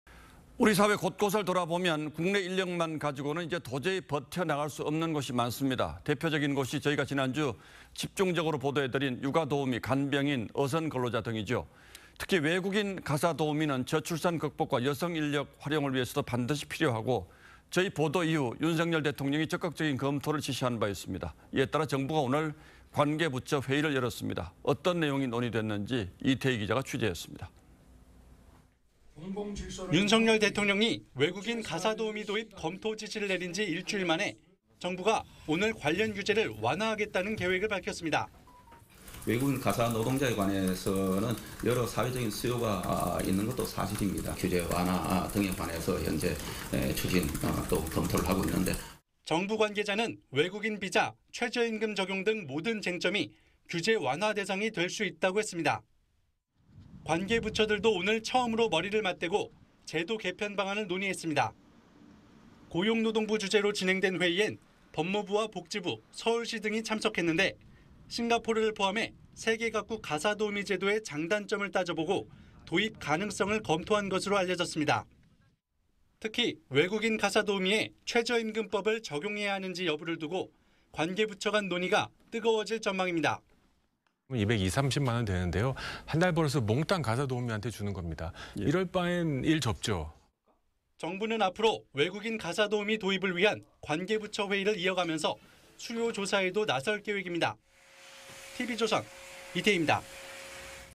추경호 "외국인 가사노동자 규제 완화 추진"
우리 사회 곳곳을 돌아보면 국내 인력만 가지고는 도저히 버텨 나갈 수 없는 곳이 많습니다. 대표적인 곳이 저희가 지난주 집중적으로 지적한 육아도우미 , 간병인 , 어선근로자 등이지요. 특히 외국인가사도우미 는 저출산 극복과 여성인력 활용을 위해서도 반드시 필요하고, 저희 보도 이후 윤석열 대통령이 적극적인 검토를 지시한 바 있습니다. 이에따라 정부가 오늘 관계부처 회의를 열었습니다.